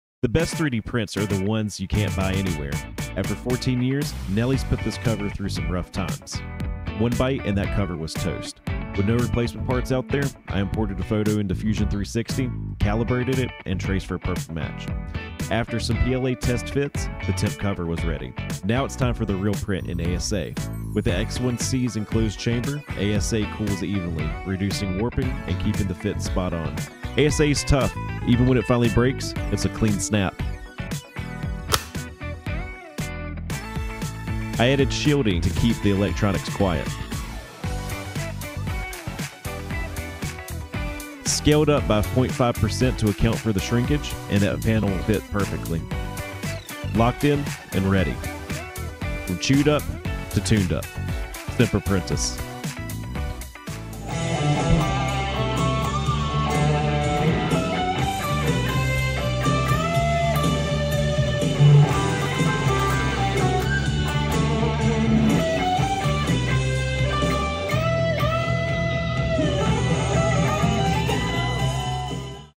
Ibanez SZ320 Electronics Cavity Cover Design and Print!
14 years with this Ibanez SZ320, and it’s been through countless gigs, mods, and memories. One afternoon during a pickup swap, Nellie nearly ended its run. She took one bite out of the electronics cover. The guitar is long discontinued, so there wasn’t a replacement to be found anywhere. 😧

That’s where the beauty of practical 3D printing comes in. I modeled the part from scratch in Fusion 360, dialed in the fit with PLA test prints, then ran the final in ASA on the X1C. The enclosed chamber kept the print stable, reducing warping so the final fit was spot‑on. Added shielding, few new screws, and the guitar was back in action.

From a chewed‑up to tuned-up! This is why I love designing and printing parts you can’t buy.